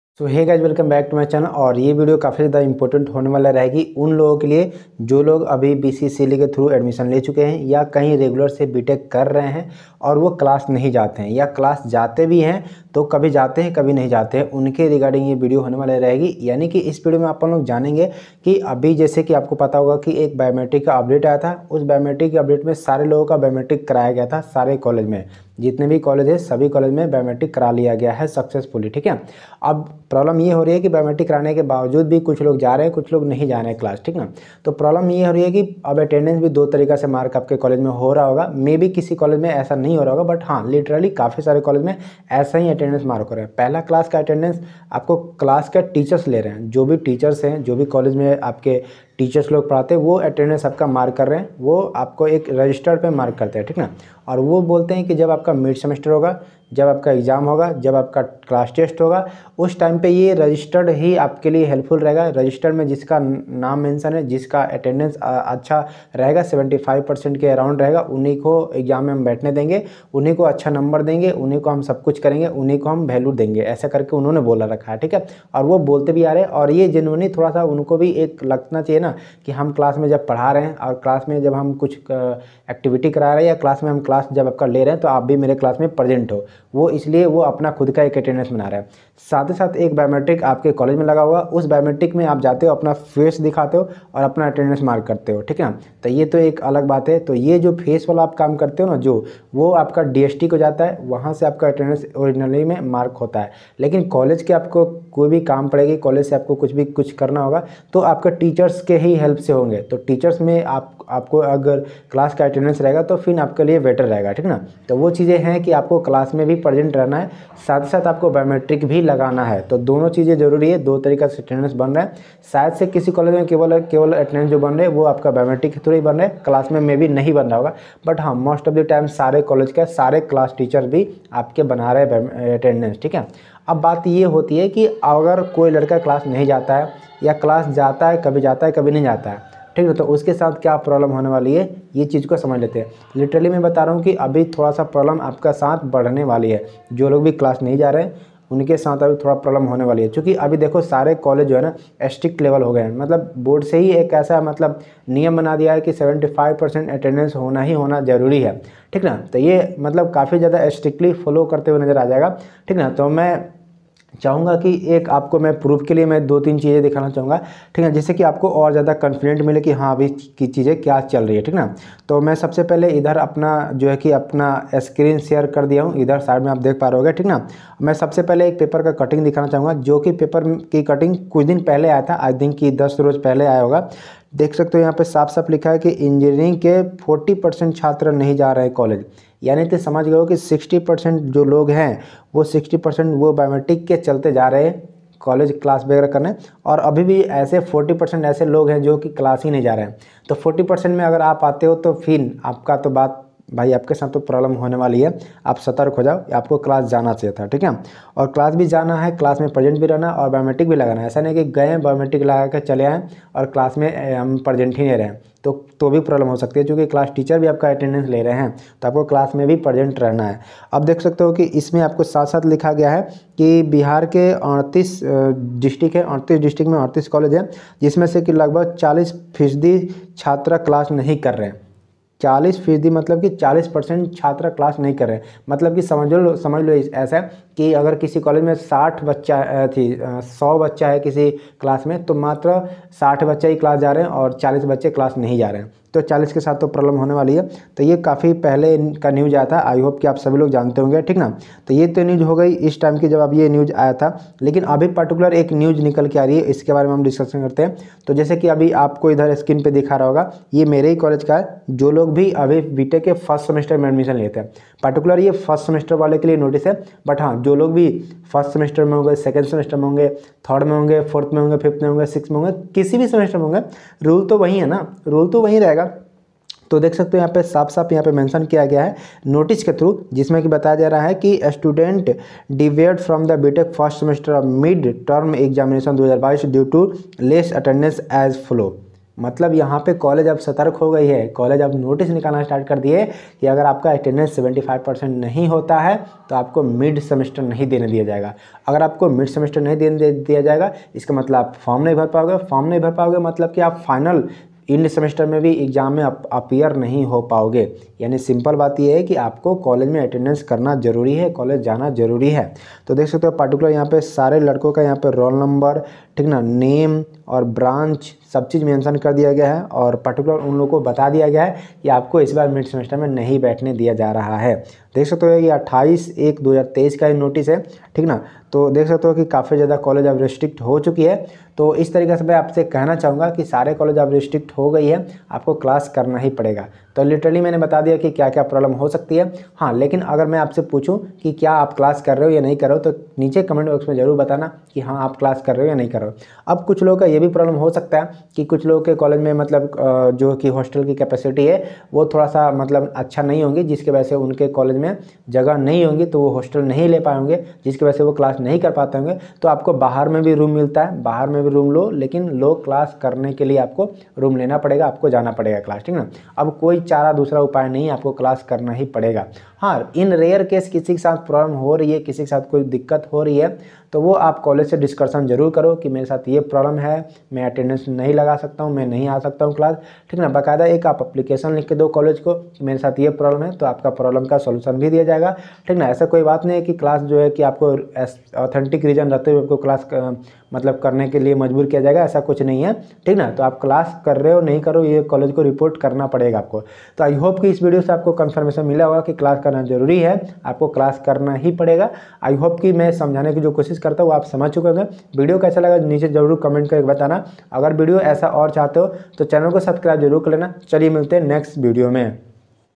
75% उपस्थिति नहीं हुआ तो क्या होगा? | AKU ALL SEMESTER BIOMETRICS ATTENDENCE NEW UPDATES 🔥🔥
75% उपस्थिति नहीं हुआ तो क्या होगा? | AKU ALL SEMESTER BIOMETRICS ATTENDENCE NEW UPDATES 🔥🔥

BCECE LE 2023 | Full Course- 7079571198